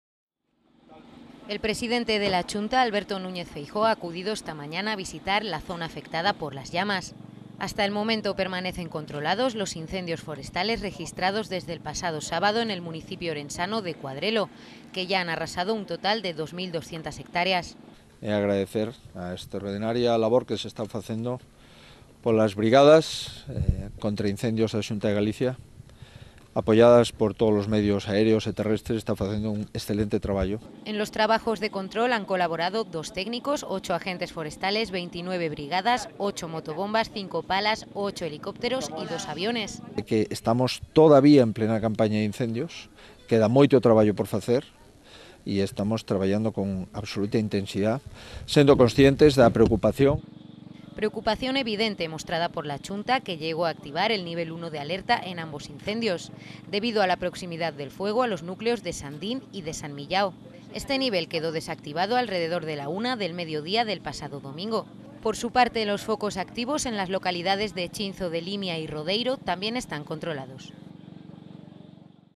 Un Feijóo "preocupado" por los incendios visita Cualedro
El presidente de la Xunta de Galicia, Alberto Núñez Feijóo, ha visitado los montes de Cualedro y Monterrei quemados durante el fin de semana por sendos fuegos originados en las parroquias de Vilela y A Xironda (ambas de Cualedro), donde ha pedido "formalmente" la colaboración de los ciudadanos para detectar "a los que producen los fuegos".